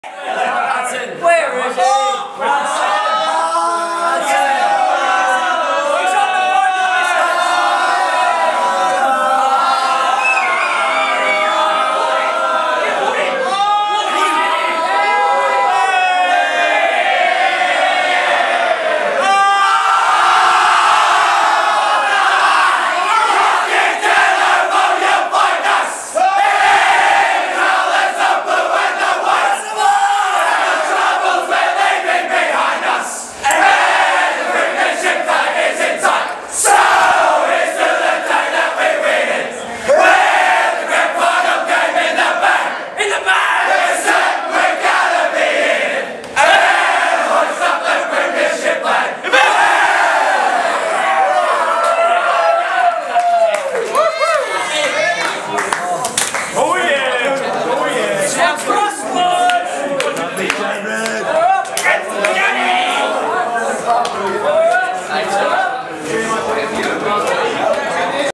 LBU Back To Back Premiership song
The Cats celebrate their 94 point HDFL premiership against North Bendigo